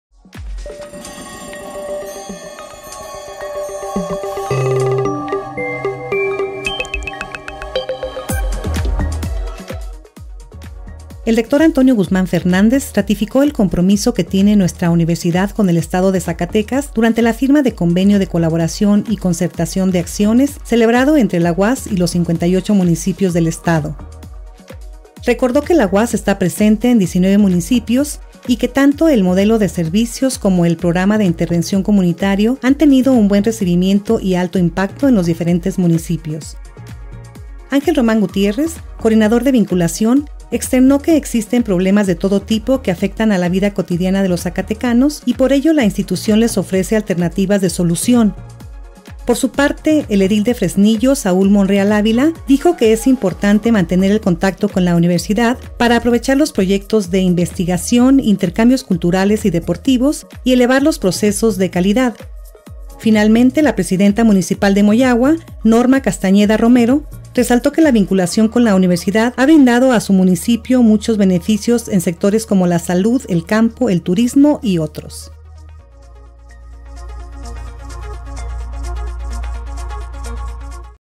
Firma convenio la UAZ con los 58 municipios del Estado de Zacatecas
El rector Antonio Guzmán Fernández ratificó el compromiso que tiene nuestra Universidad con el estado de Zacatecas durante la “Firma de convenio de colaboración y concertación de acciones” celebrado entre la UAZ y los 58 municipios del Estado.
 Recordó que la UAZ está presente en 19 municipios, y que tanto el modelo de servicios como el programa de intervención comunitario han tenido un buen recibimiento y alto impacto en los diferentes municipios.
 Ángel Román Gutiérrez, coordinador de Vinculación, externó que existen problemas de todo tipo que afectan a la vida cotidiana de los zacatecanos, y por ello la institución les ofrece alternativas de solución.
 Por su parte, el edil de Fresnillo, Saúl Monreal Ávila, dijo que es importante mantener el contacto con la universidad, para aprovechar los proyectos de investigación, intercambios culturales y deportivos, y elevar los procesos de calidad.
 Finalmente, la presidenta municipal de Moyahua, Norma Castañeda Romero, resaltó que la vinculación con la Universidad ha brindado a su municipio muchos beneficios en sectores como la salud, el campo, el turismo, y otros.